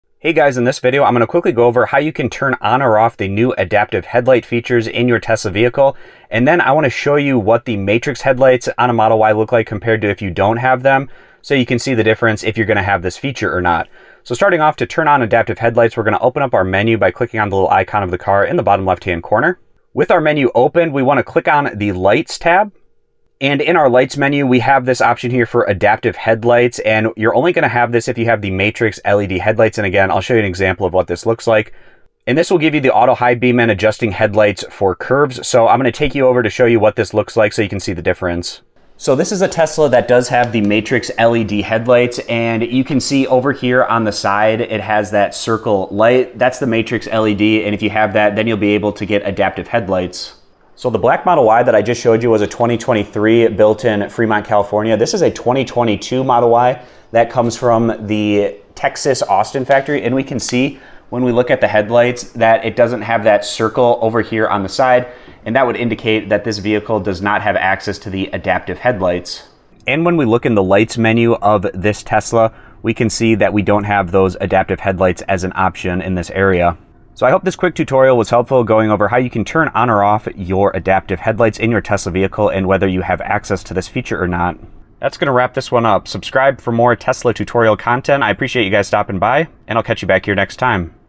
Tesla How To Turn On Adaptive Headlights - How To Tell Difference Matrix LED vs. Standard Headlights
Hello, In This Video I Go Over Tesla How To Turn On or Off Adaptive Headlights - How To Tell Difference Matrix LED vs. Standard Headlights in Your Model 3 Model Y Model S Model X Cybertruck Example Step by Step Instructions, Guide, Tutorial, Video Help

In this video I walk through the step by step guide, instructions, tutorial on how to turn on or off adaptive headlights in your Tesla Model 3, Model Y, Model S, Model X, Cybertruck. I Hope This Video Helped!